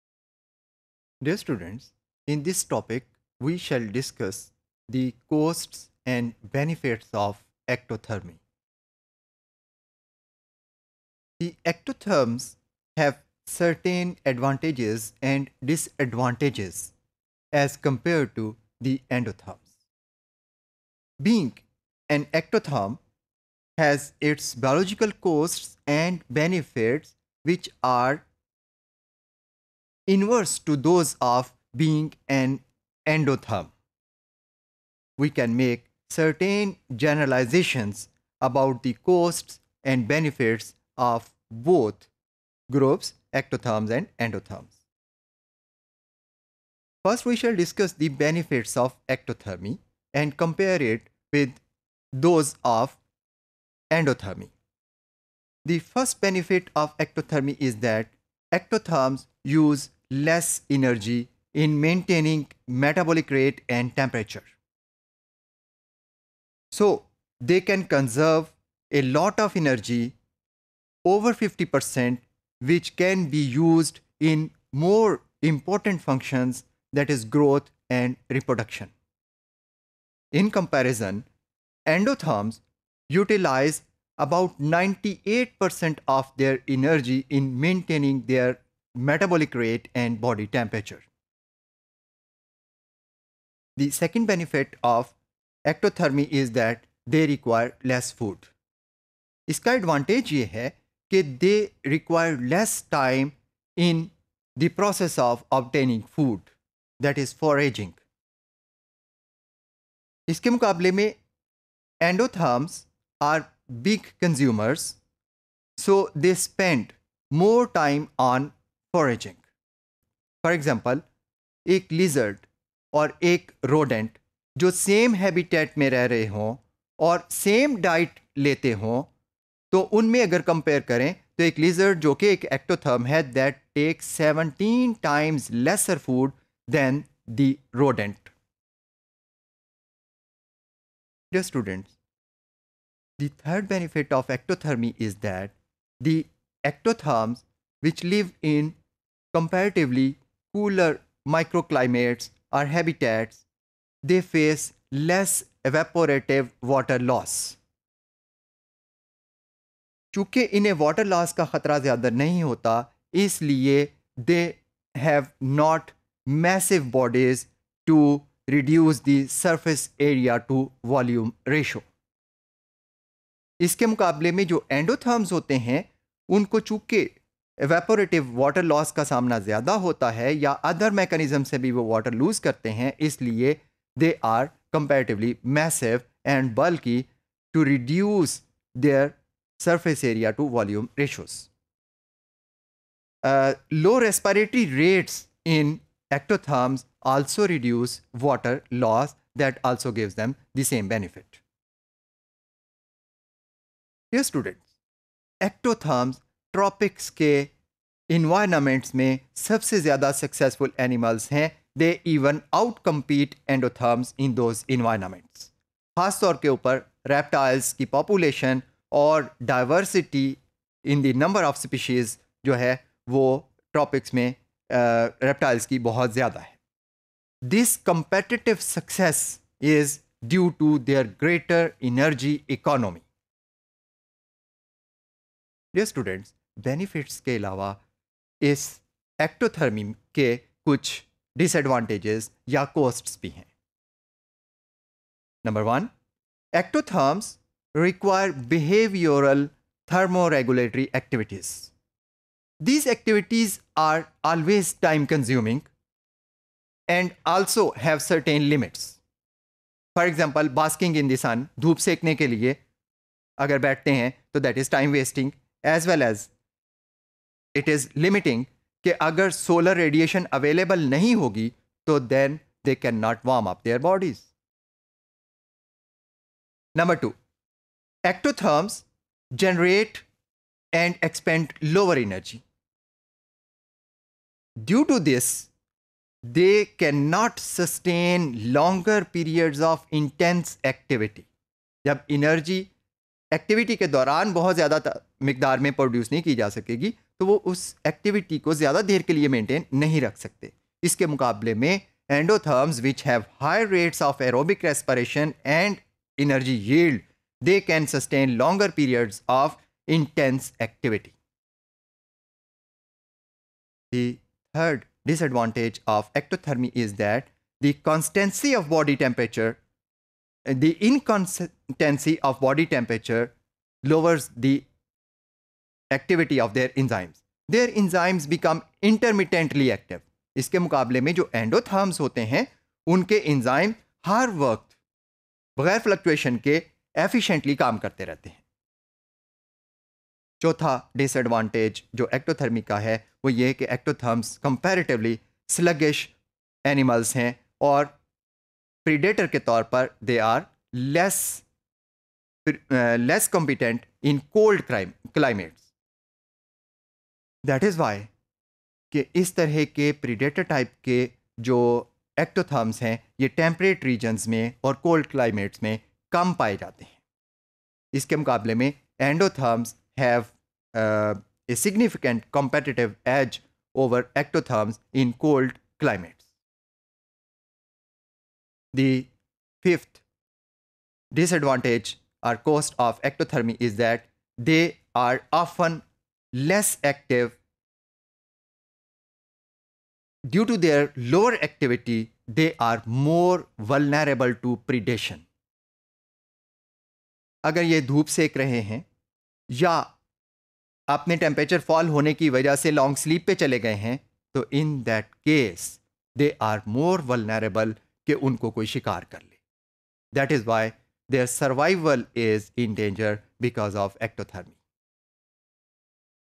Costs and Benefits of Ectothermy | Animal Physiology and Behavior (Theory) | ZOO502T_Topic222
Topic222: Costs and Benefits of Ectothermy,
By Dr. Ateeq ul Jaleel